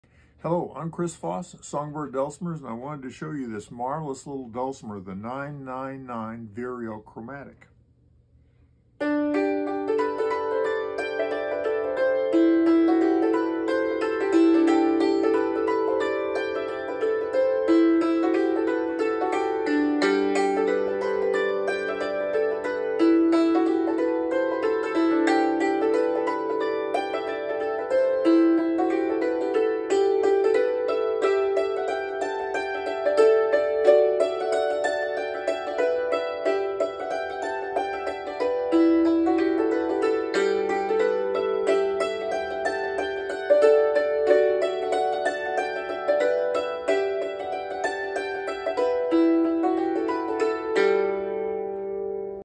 Introducing the new 9/9/9 Vireo Chromatic hammereddulcimer Best travel dulcimer on the market!
Looking for a dulcimer that is small and light that you can tote around wherever you go? You’ve found it. The Vireo Chromatic hammered dulcimer is the best travel dulcimer on the market today! It’s ultra-portable, capable, great sounding, stable, and of course lots of fun!

Features:

9/9/9
Length: 31 1/2″  Width: 11″  Height: 3 1/2″ including the bridges
Weights a little over 6 pounds
Top is Quarter Sawn Douglas Fir and the back is Pine plywood
Pinblocks are Hard Maple, the rails and bindings are Cherry, and the bridges are Walnut
Comes with a backpack case, altogether weighs only 7 to 8 pounds
Comes with a playing leg that screws into the back
Perfect intermediate/advanced travel dulcimer!
Comes with hammers, tuning wrench, tuning diagram, playing leg and backpack case